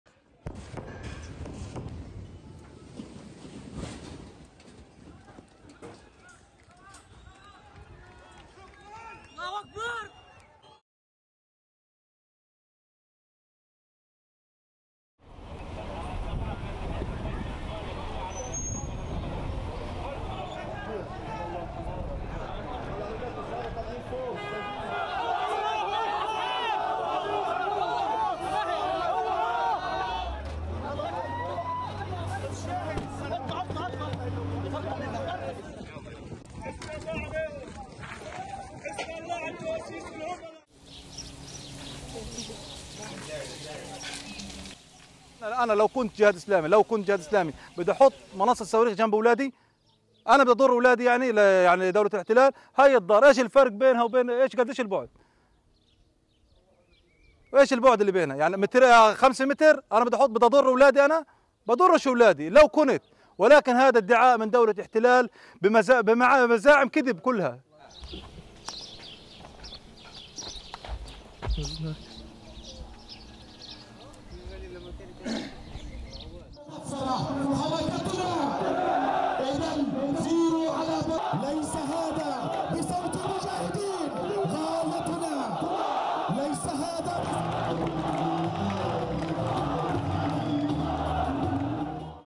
Israeli fighter jets strike targets in the Gaza Strip
Israeli fighter jets struck targets in the Gaza Strip and Palestinian militants fired rockets toward Jerusalem on Friday, further escalating the most violent confrontation in months between Israel and militants in the Gaza Strip despite efforts to broker a cease-fire. (May 12)